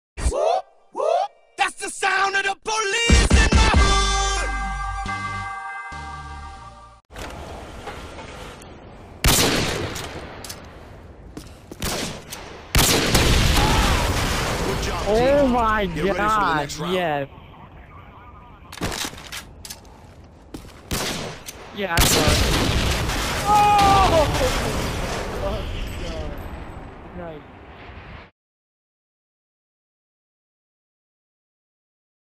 What a Tease!
just a quick clip of what i thought was a good killcam and even the kids on the other team thought i shot him .. i thought it was a lil funny .. Sick killcams coming soon get ready for Ep.5